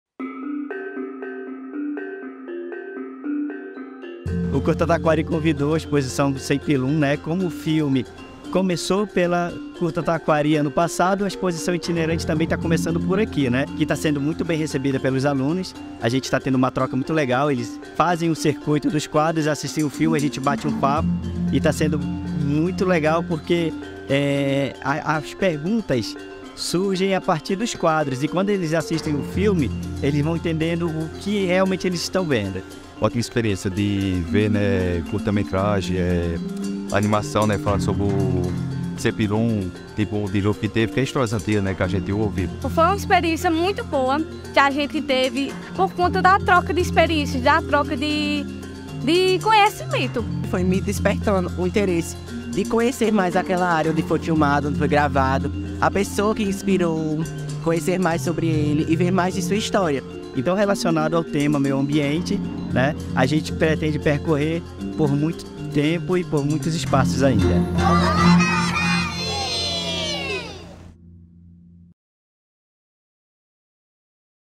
Exposição Cem Pilum - a História do Dilúvio | 16 º Curta Taquary
O Museu Amazônico levou até Pernambuco a exposição Cem Pilum - a História do Dilúvio de Thiago Morais.
Salão Paroquial - Igreja Matriz de Santo Amaro
Março 2023